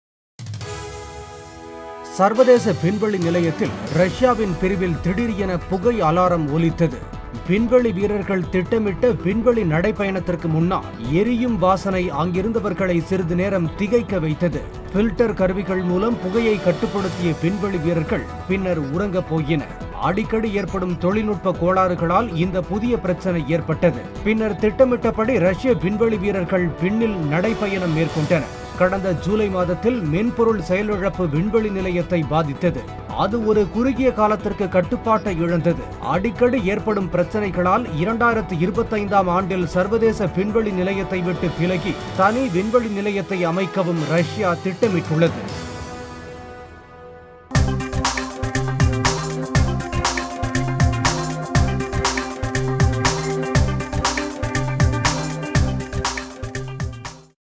சர்வதேச விண்வெளி நிலையத்தில் திடீரென புகை அலாரம்..!
மென் பொருள் செயல்பாட்டில் கோளாறுகளால் ஏற்படும் பிரச்சினைகள்